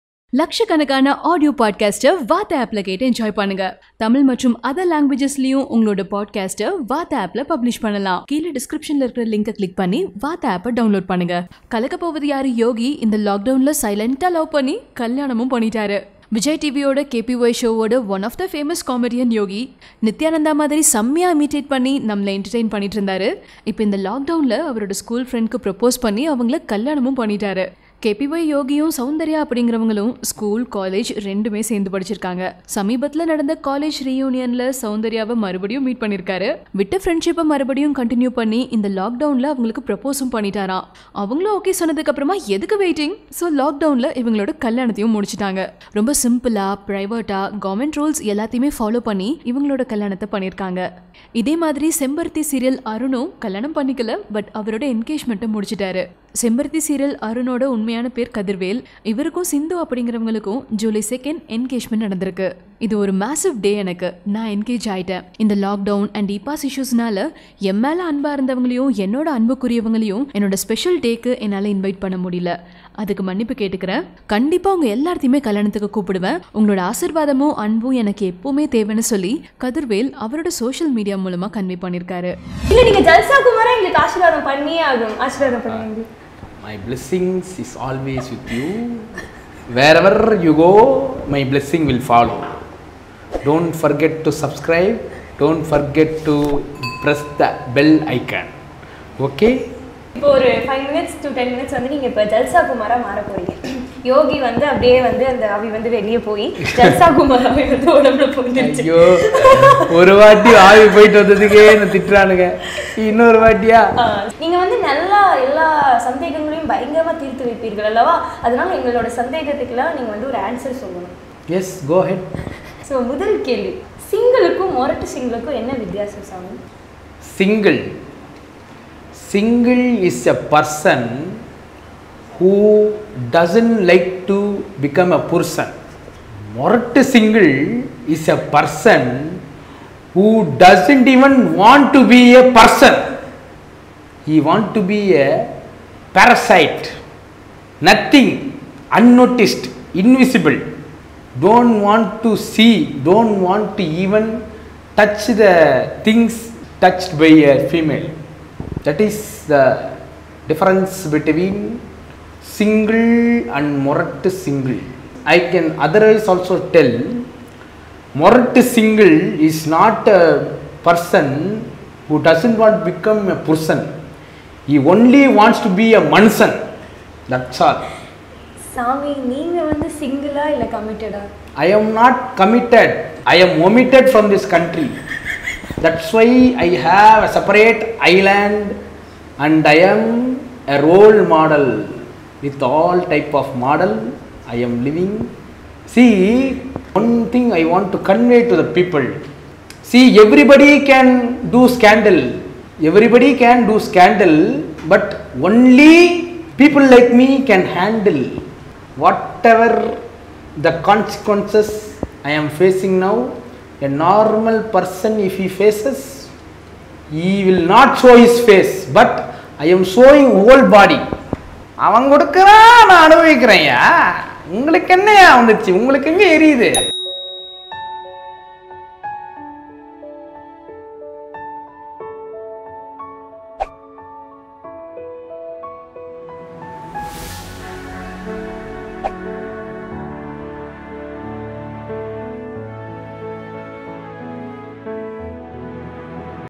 காதல் திருமணம் செய்த Kpy Yogi | Marriage, Jalsa Kumar Comedy, Nithyananda, Vijay Tv | Tamil News
or install the Vaarta Android app!
Actor Yogi has hit it big after his recent turn as absconding controversial godman Nithyananda in Kalakka Povathu Yaaru on Vijay TV. Now Yogi has been cast in the role again in the upcoming movie 'Polladha Ulagil Bayangara Game' (PUBG). As said by the team, Yogi's performance in PUBG will be an extension of his act in Kalakka Povathu Yaaru show, and that the actor has made use of popular funny lines of Nithyanandha (No Soodu No Suranai - The me in the me) in an even funny and entertaining way in the scenes. Yogi's Nithyanandha act as JalsaKumarcomedy become so viral.